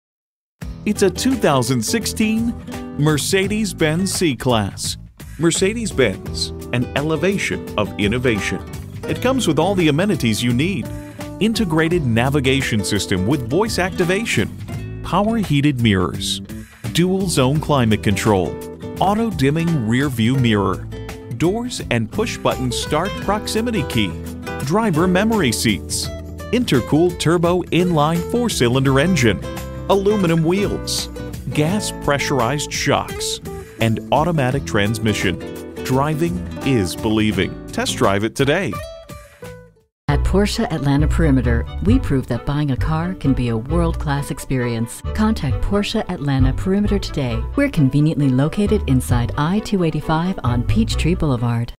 Used 2016 Mercedes-Benz C-Class Atlanta, GA SLP14840A - SOLD
Year: 2016
Make: Mercedes-Benz
Model: C-Class
Trim: C 300
Engine: 2.0L I4 Turbocharged
Transmission: 7G-TRONIC PLUS 7-Speed Automatic
Color: Silver
Interior: Black
Mileage: 47327
Stock #: SLP14840A
VIN: 55SWF4KB6GU173630
Iridium Silver Metallic $720brPanorama Sunroof $1,480brLed HeadlampsbrLED TaillightsbrHeated Front Seats $580brHeated Steering Wheel $250brBlind Spot Assist $550brIlluminated Star $550brPremium Package: Sirius XMA® SatellitebrRadio W/ 6 Mos. Service,br$2,220brMultimedia Package: High Resolution 8.4"brColor Display, ComandA®br$2,230brSport Package: Sport Bodystyling, SportbrFront Brakes Withbr$2,175brRear Deck Spoiler $350brRearview Camera $460brBlack Ash Open Pore Wood $325brbrWe look forward to hearing from you. Advertised price includes $599 Dealer Service Fee.

Address:
4006 Carver Drive